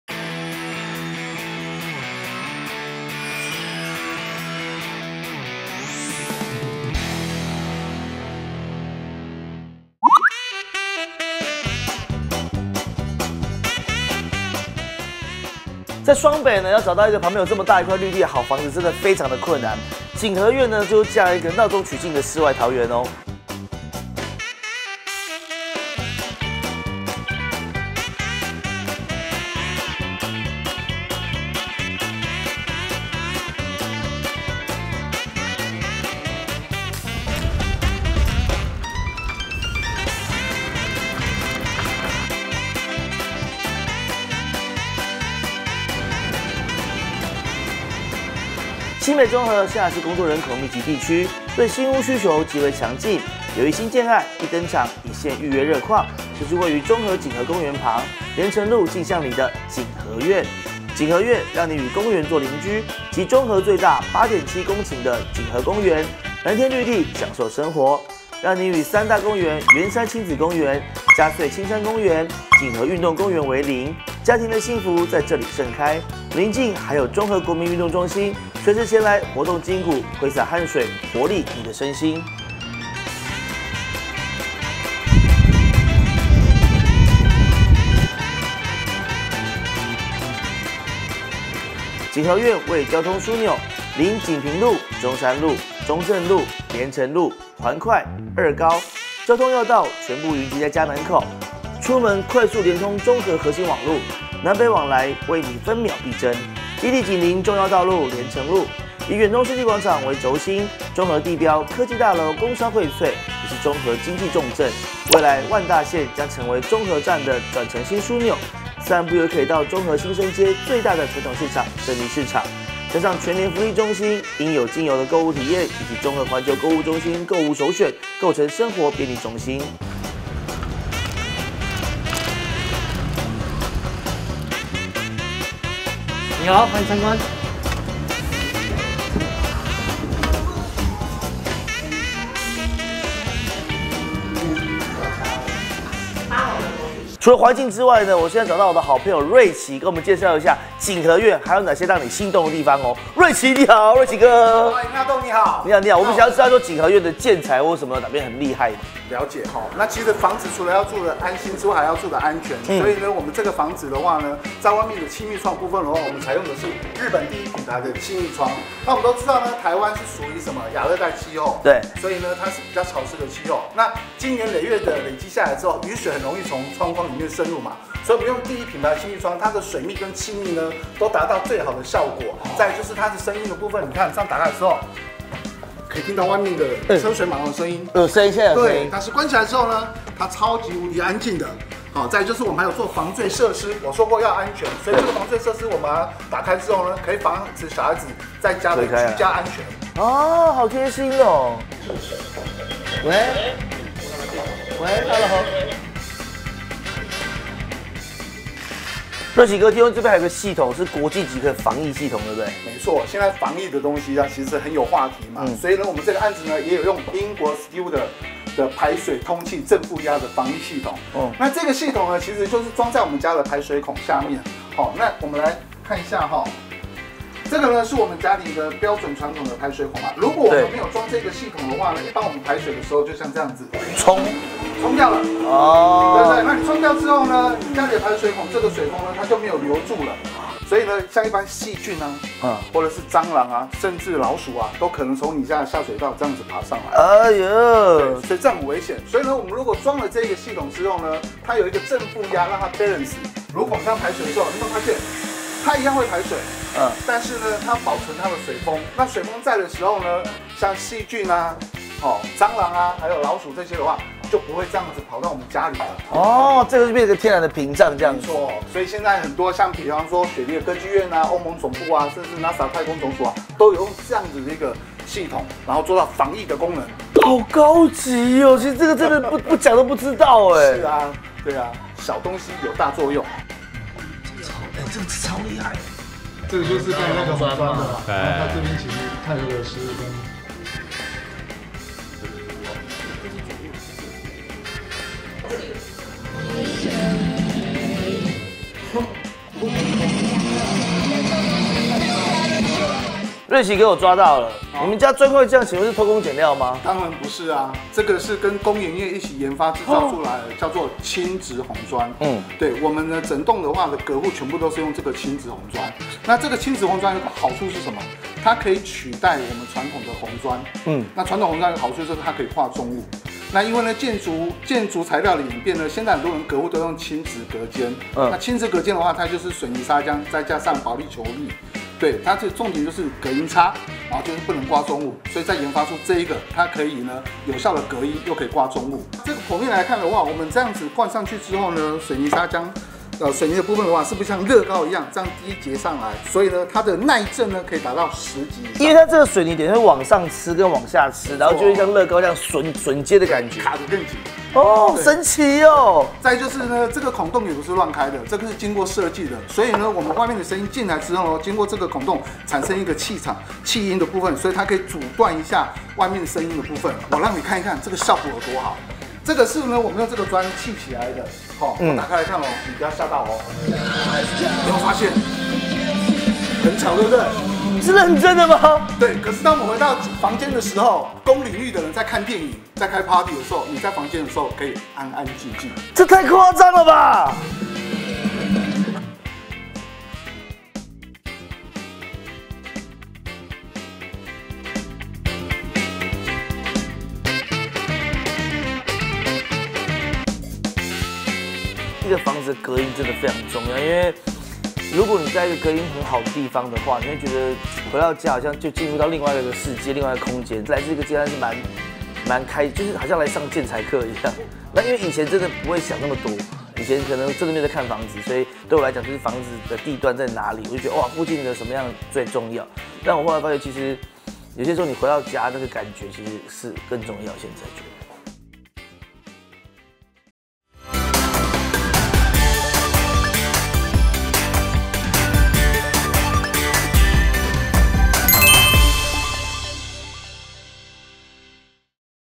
納豆相信第一直覺 買房最重隔音其次是材質 地產天團 | 台灣新聞 Taiwan 蘋果新聞網
「蘋果娛樂地產天團」大使納豆介紹位於中和的「錦和苑」建案，他買房第一最重視隔音，其次是材質，如果隔音好，會讓他感覺回家像進到另外一個世界。以前他在東區租房子時還特地再加裝一層氣密窗，有了2層氣密窗的防護，整間房子變得超級安靜，「如果你窗簾弄得夠暗，有時候早上起來真的不知道自己身在什麼地方，但就是適合休息」。

最近正在看屋的納豆開始了解每個建案主打的材質，以前對他來說買房只有地段第一，後來他漸漸發現回到家的感覺也很重要，提到採光和格局，納豆說喜歡房子雙面都有採光，也要格局方正，不過他最相信自己走進去房子的第一眼直覺，「因為不一定每間房子都可以面面俱到，所以自己的感覺蠻重要的」。